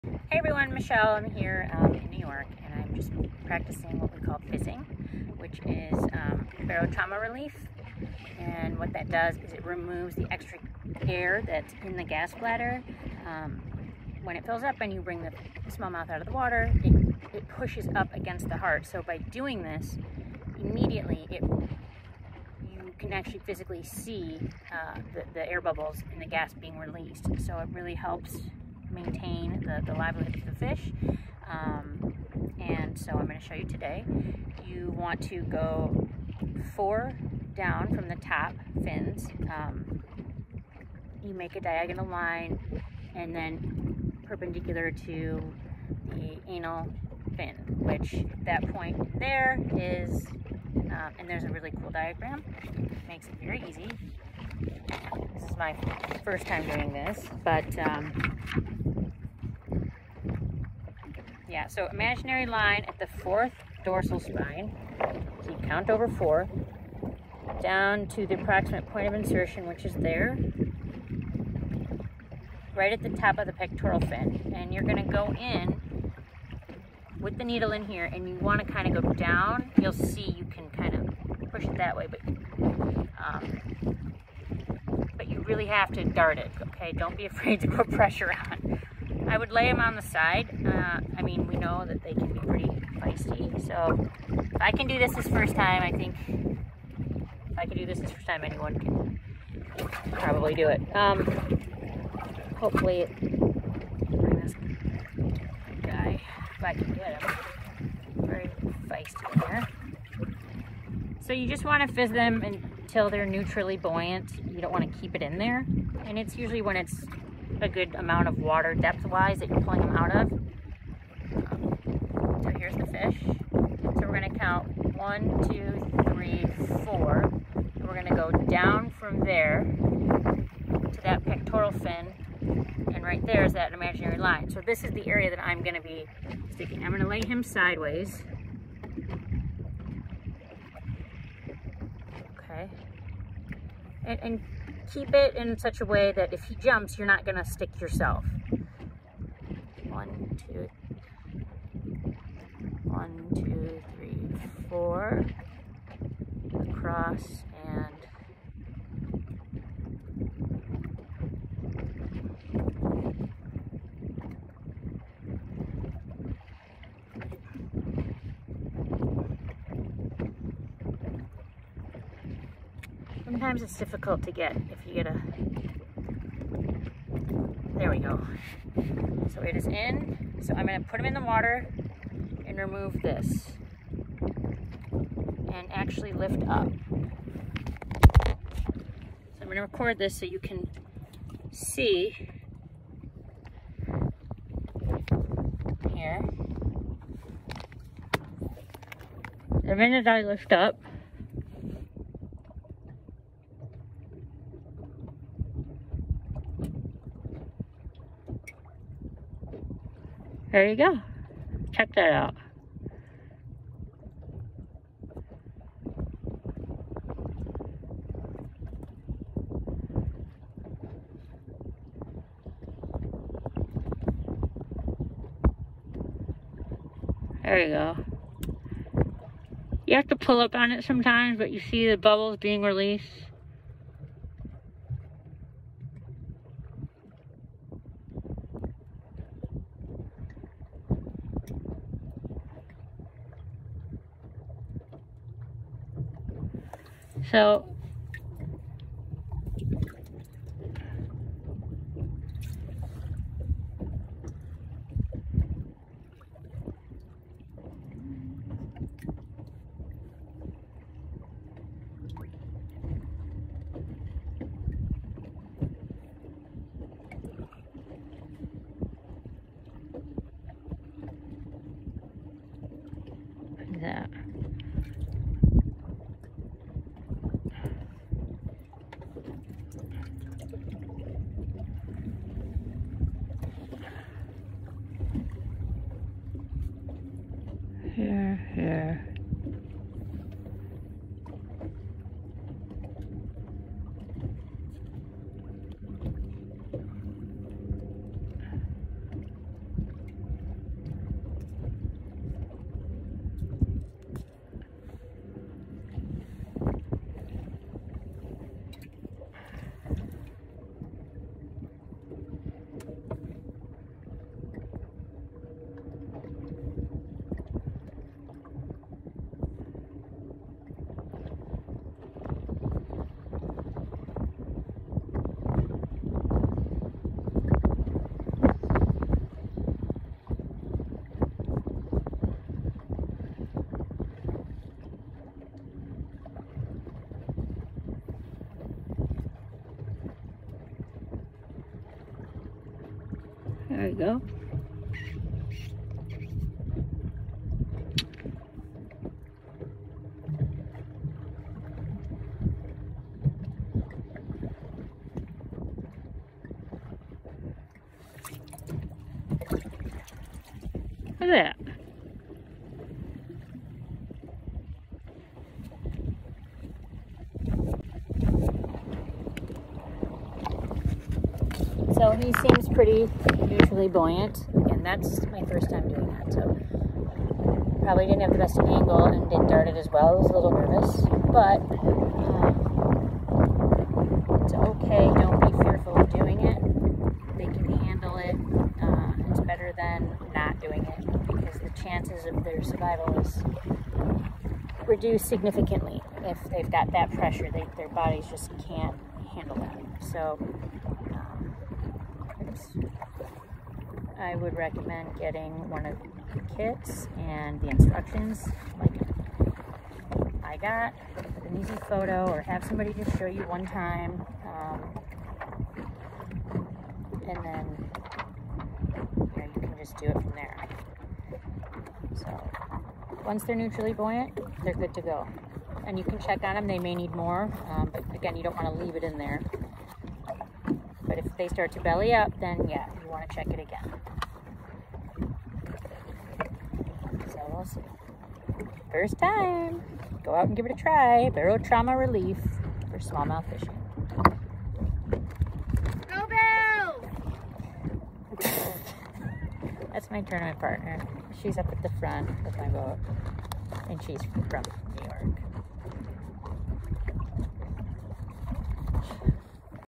Barotrauma Relief. How to “fizz” smallmouth bass.
A short video of a first time “fizzing” a smallmouth bass. This type of Barotrauma Relief helps to reduce stress, pressure and ultimately ensure the health and longevity of the fish. Although it may seem intimidating I demonstrate my first time fizzing in hopes others will feel more comfortable trying it on their own. Bass when taken from deeper waters typically 20-30 ft typically require this treatment. Many people do not realize that during a catch and release situation simply throwing them in the Live well will not guarantee longevity once returned to the water. The gas bladder is full of air and too much pressure pushes up against the heart of the fish which will not sustain their well being even with well aerated and proper temperatures alone. This has become a regular practice in my catch and release fish care maintenance. All the techniques used in conjunction will help the bass to return healthy and continue on. Enjoy and do not be afraid to explore and educate!